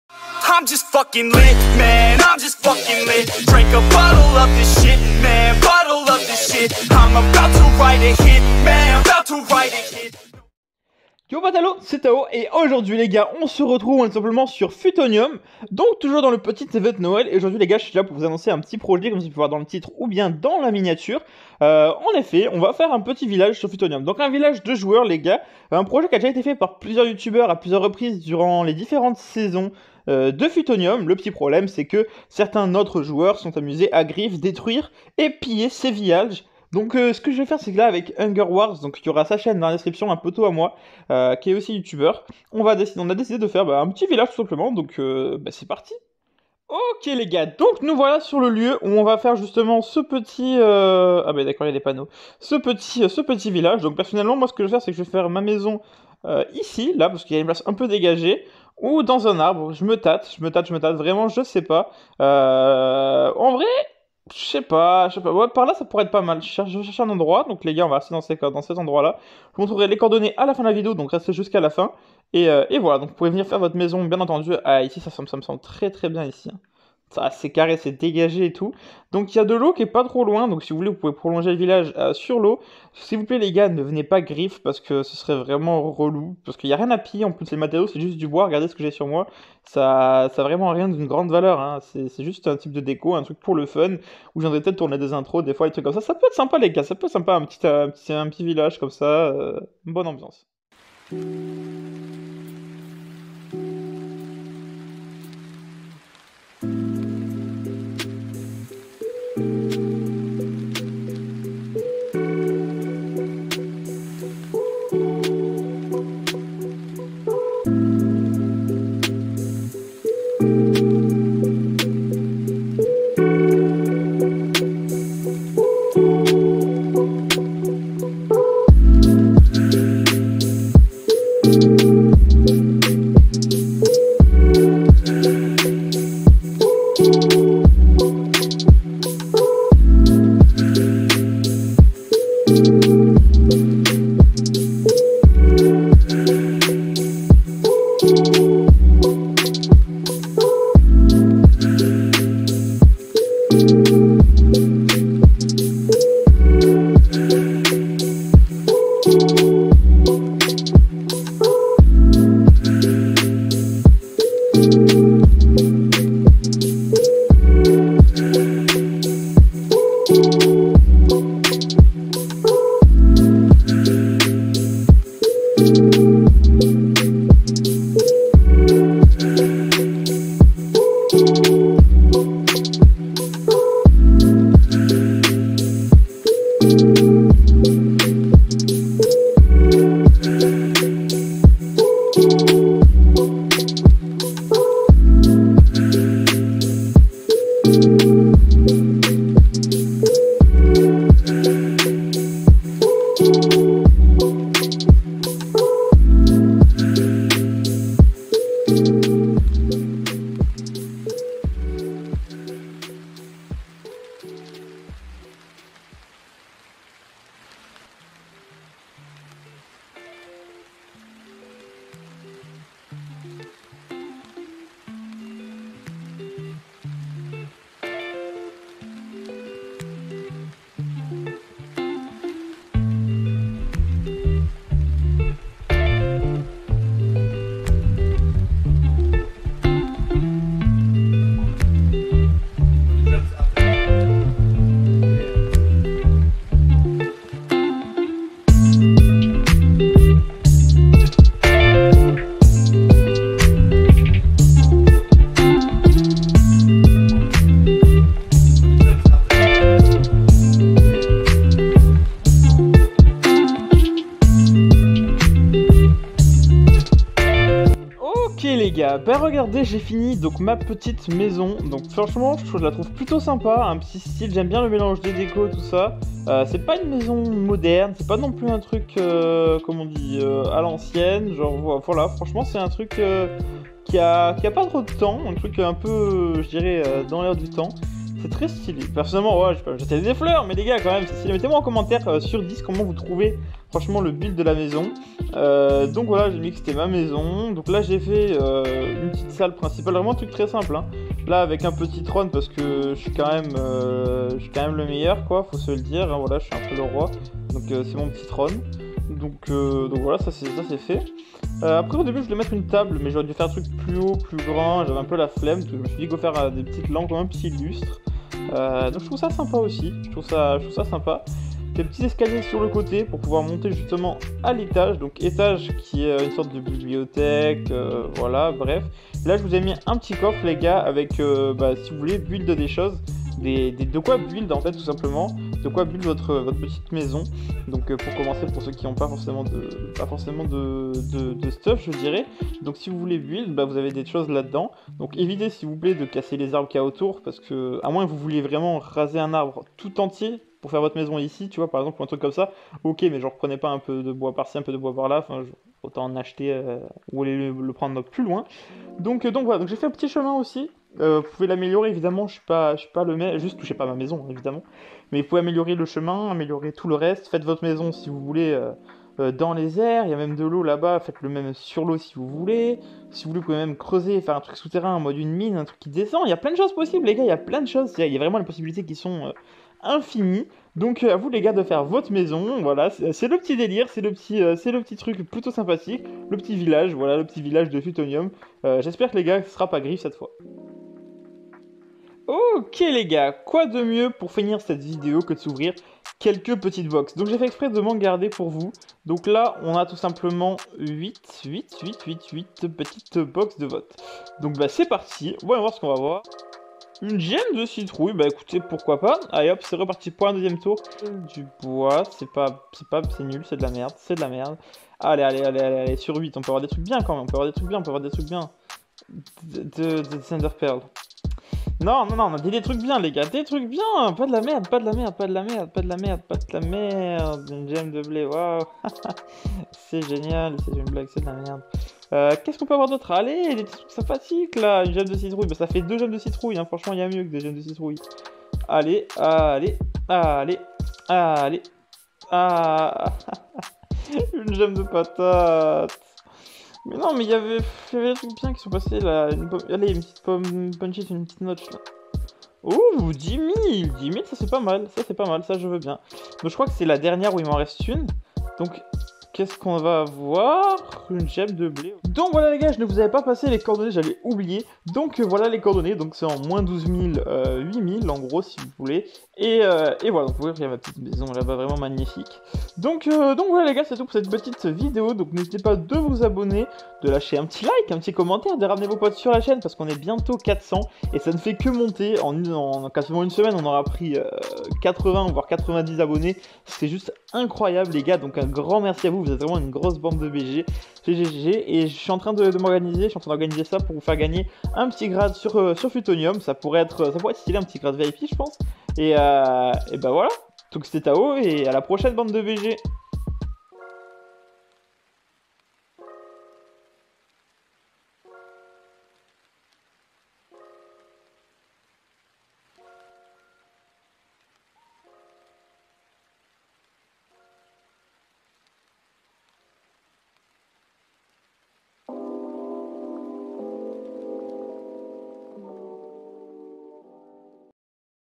Je construit un village pour vous !
Aujourd'hui c'est mon anniversaire !
Et. J'ai décidé de relancer un projet, ceului d'un village de joueurs !
Alors à vos build ça on a un village à bâtir !

Bon visionnage bande de bgs ;)